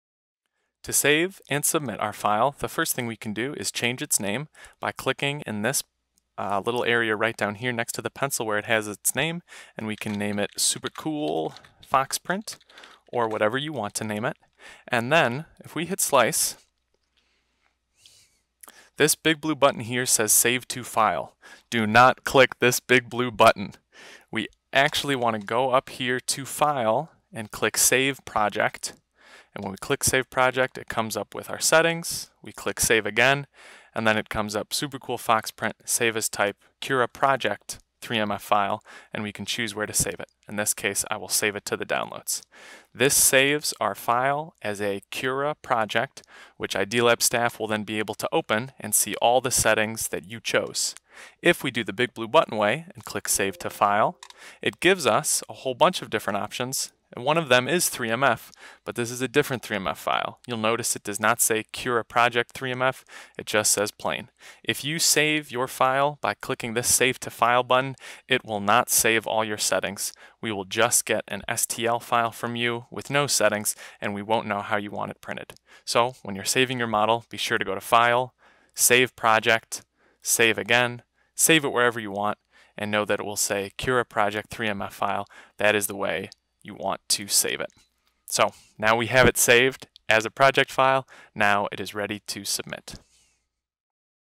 Learn to 3D Print: Saving a Cura Project File
This video is part of the Learn to 3D Print class at the Idea Lab in the Erie County Public Library. This class certifies you to have items printed at the Idea Lab if you are an Erie County Public Library cardholder.
The Idea Lab uses UItimaker 3 and S3 printers.
The slicing software we use is Cura.